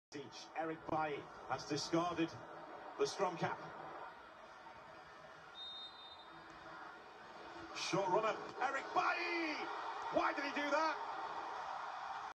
Eric Bailly crazy penalty miss against Egypt at AFCON2021/2022 edition
Eric Bailly just missed a decisive penalty for Ivory Coast which sent Mo Salah Egypt straight to the next round of the AFCON